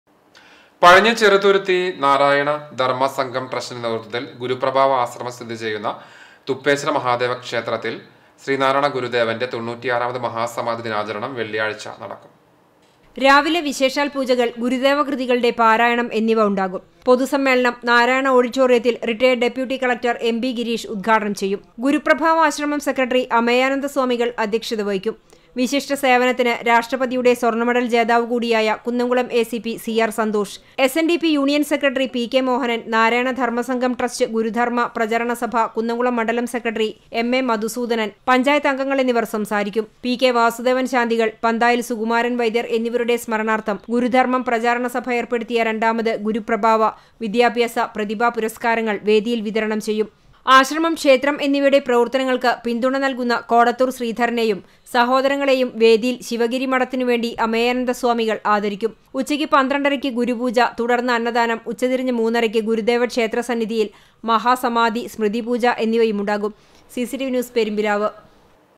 ശ്രീ നാരായണ ഗുരുദേവന്റെ 96- മത് മഹാസമാധി ദിനാചരണം വെള്ളിയാഴ്ച നടക്കും.
പഴഞ്ഞി ചെറുതുരുത്തി-പെരുന്തുരുത്തി പെരുന്തുരുത്തി ശ്രീ നാരായണ ധര്‍മ്മ സംഘം ട്രസ്റ്റ് നേതൃത്വത്തില്‍ ഗുരുപ്രഭാവ ആശ്രമം സ്ഥിതി ചെയ്യുന്ന ശ്രീ തുപ്പേശ്വര മഹാദേവക്ഷേത്രത്തില്‍ ശ്രീ നാരായണ ഗുരുദേവന്റെ 96- മത് മഹാസമാധി ദിനാചരണം വെള്ളിയാഴ്ച നടക്കും.